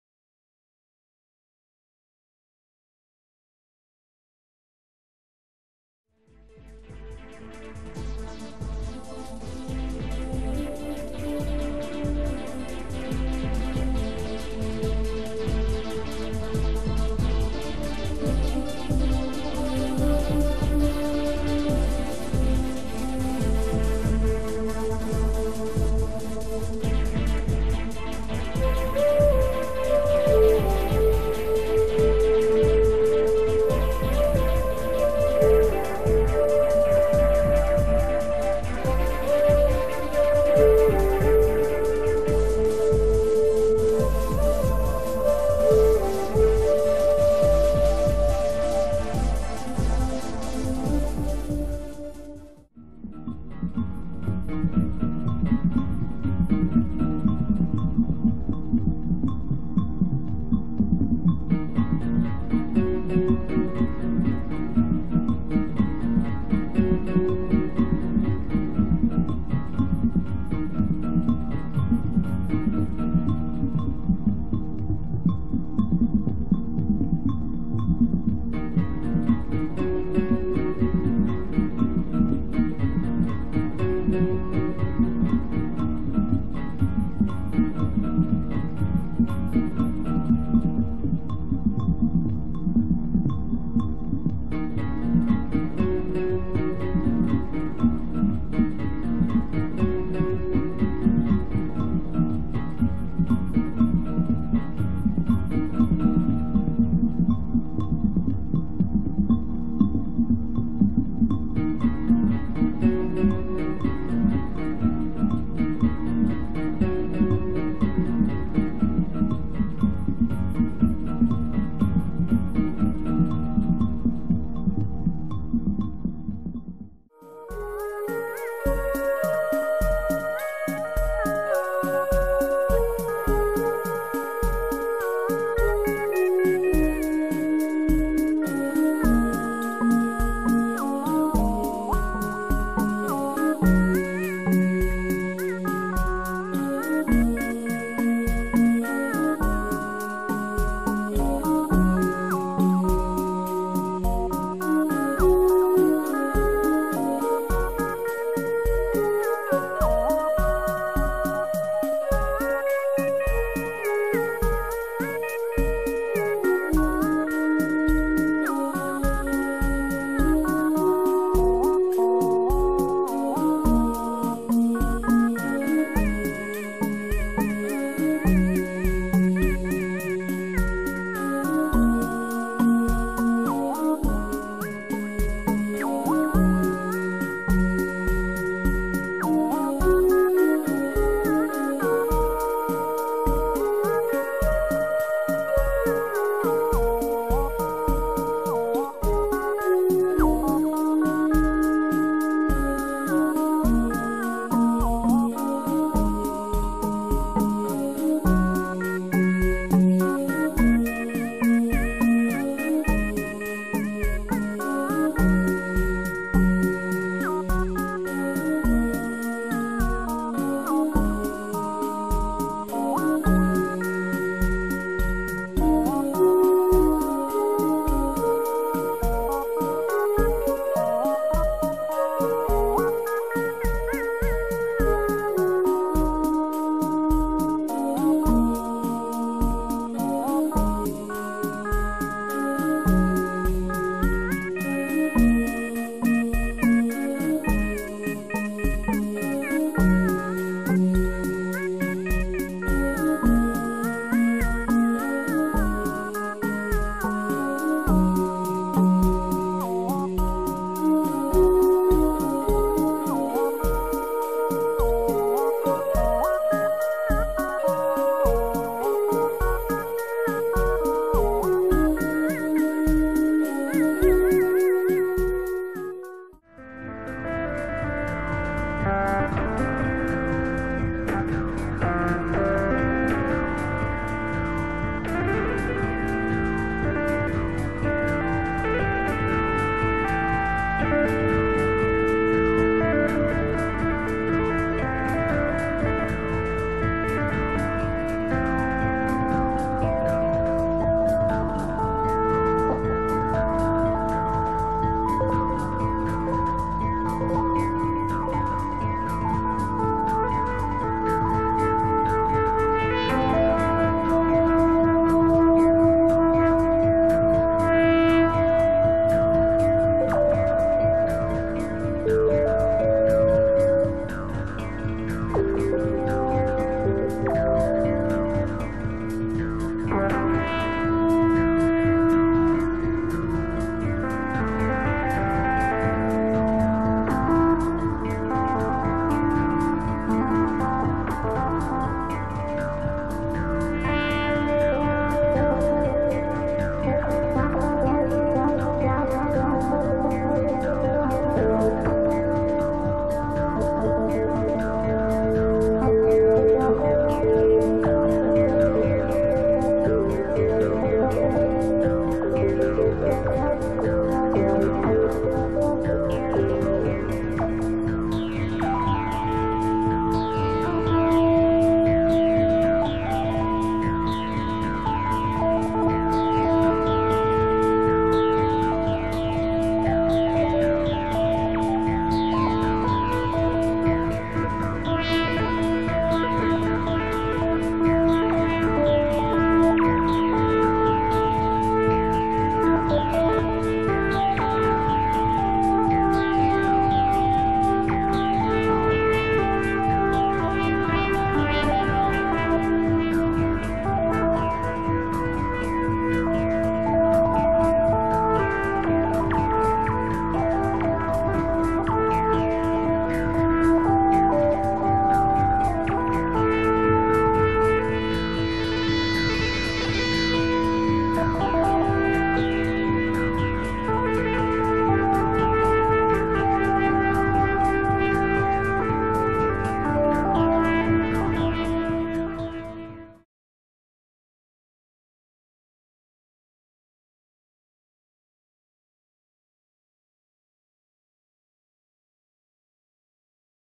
Spectacles du XXI° siècle "Danse Musique Video" pour animer un espace public
Extraits de créations en Danse Musique Vidéo - Réalisation Chantal Cavenel, Augustin Colombani, Patricia Girard-Boëx Attitude-Vibrations, Martine Kaisserlian, Gianin Loringett Off Jazz et les danseurs